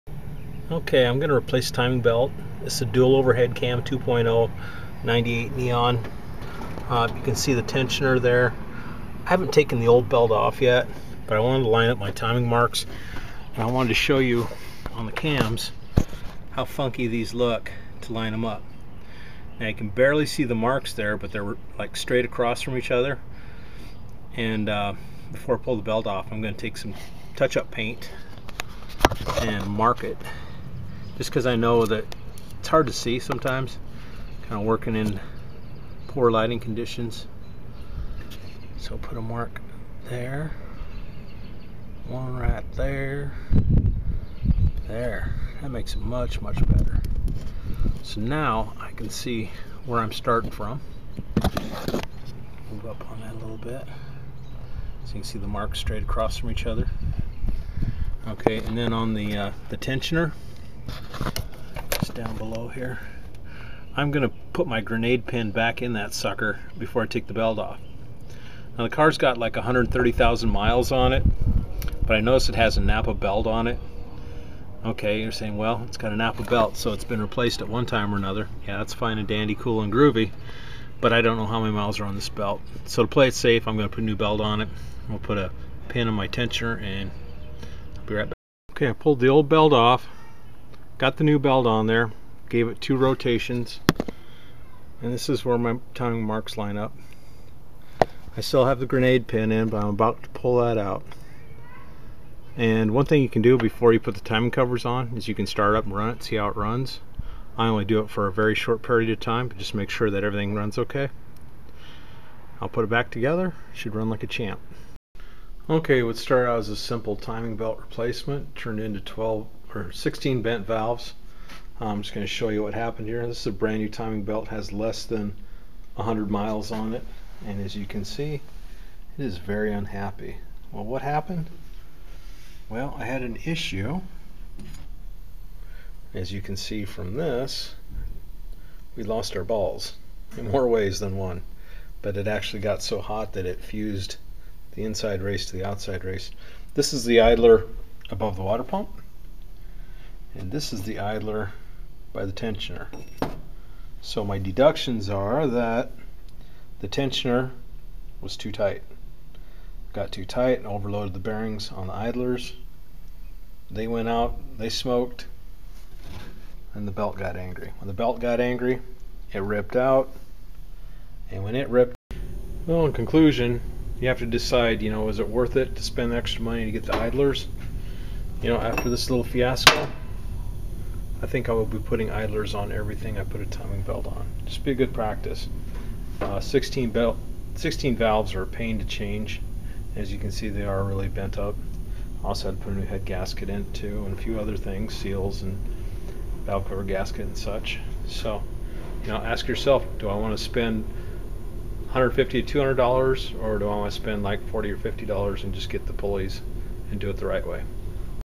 2.0 Neon DOHC timing belt aka 16 bent valves
It started as a typical DOHC timing belt replacement and within 100 miles it was time for a new set of valves. Learn from my mistake and prevent youself from repeating it. I didn't replace the idler and tensioner pulleys and it bit me hard. I set the belt tension according to service info and it was too much for the old bearings in the old pulleys. The bearings burned up and locked up and destroyed the timing belt which in turn bent all the valves. Replacing all the valves was a pain which could have been repvented by installing new pulleys.